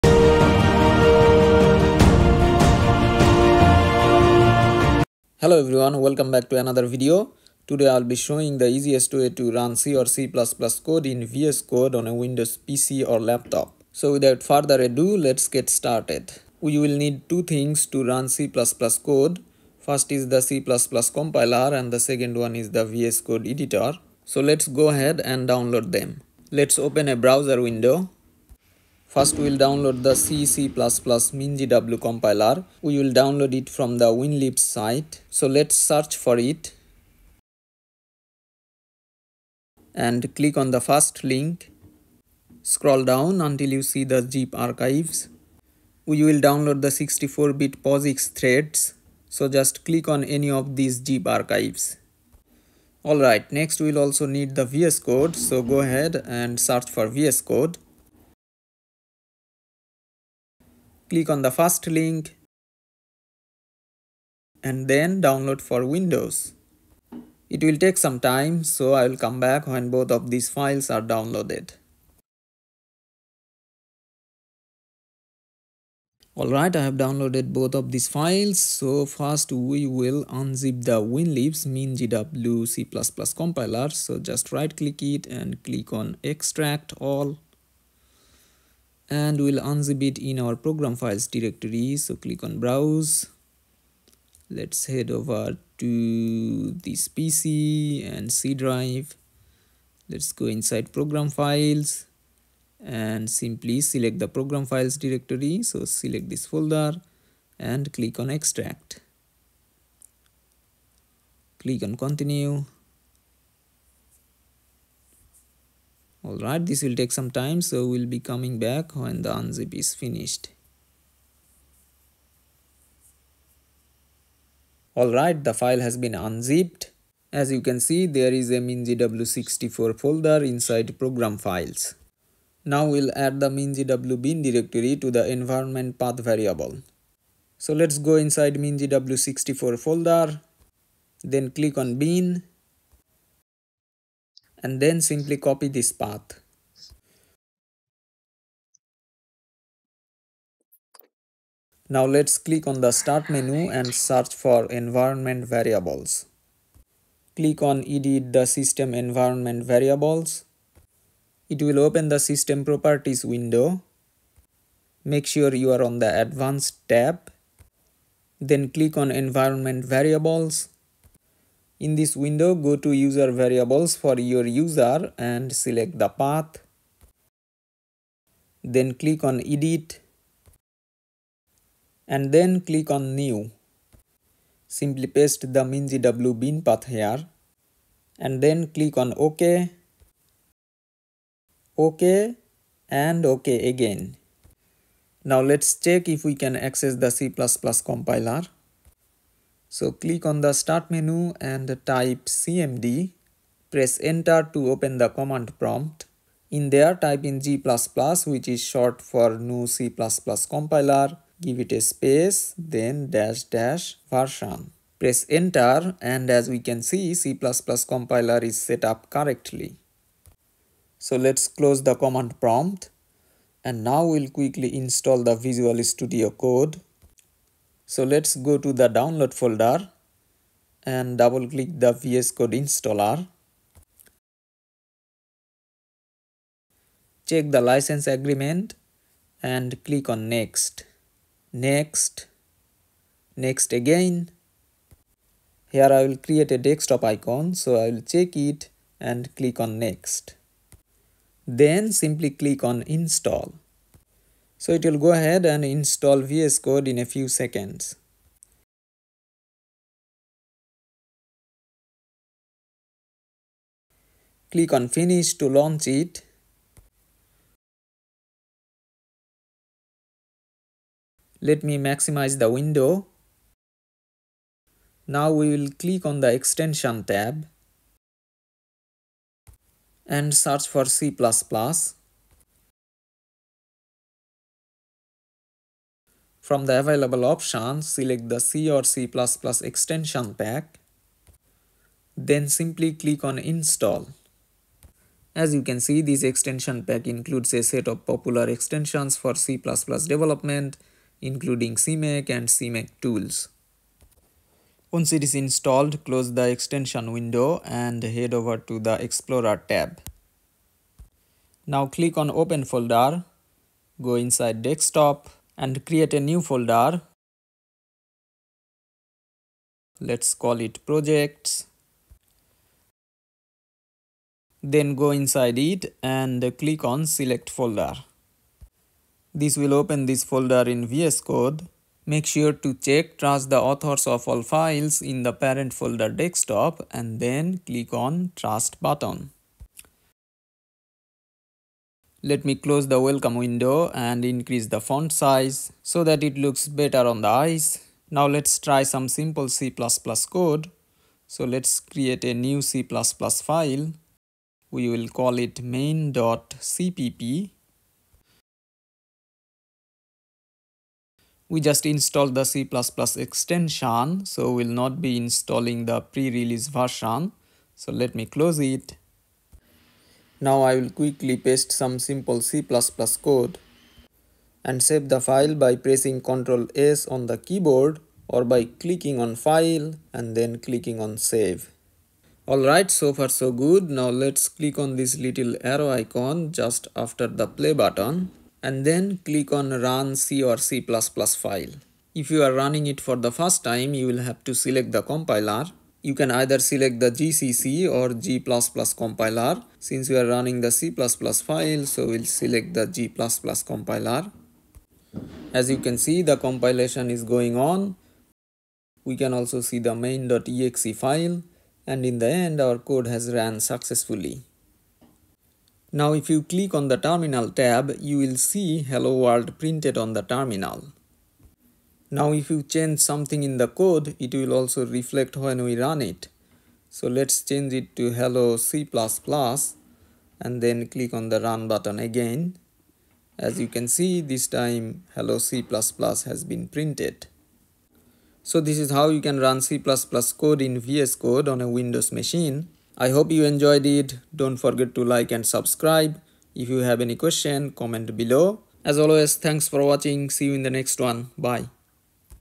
The Easiest way to Run C/C++ in VS Code on Windows 11/10
Hello Everyone,
In this video, I have shown how you can easily run C/C++ code in VS Code on Windows 11/10 Operating System.